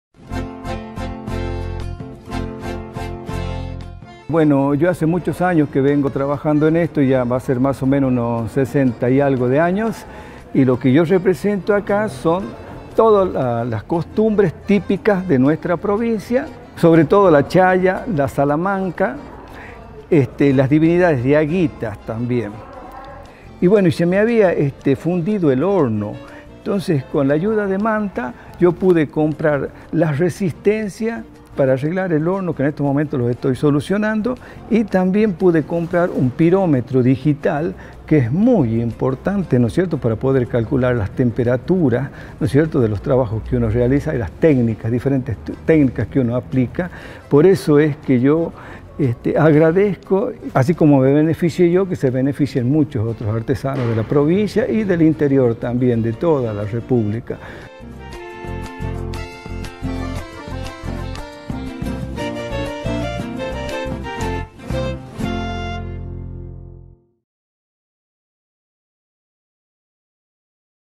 Jorge Rey, beneficiario del programa Manta
"Agradezco que me beneficie yo y también otros artesanos del país" expresa Jorge Rey, de la provincia de la Rioja quien fue beneficiario del programa Manta, con el que pudo arreglar su horno y equiparse para seguir realizando sus artesanías que representan costumbres riojanas típicas.